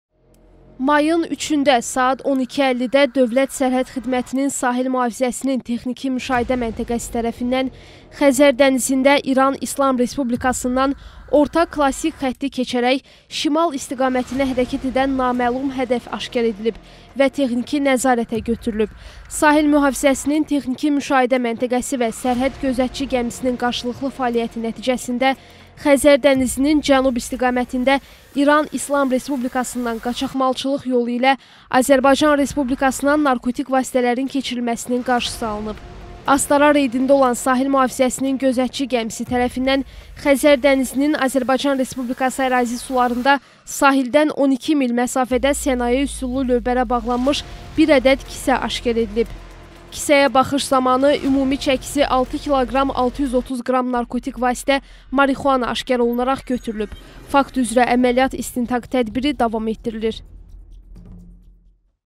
DSX-nin Xəzərdə narkotik əməliyyatı
İrandan qaçaqmalçılıq yolu ilə narkotik vasitə daşınmasının qarşısı alınıb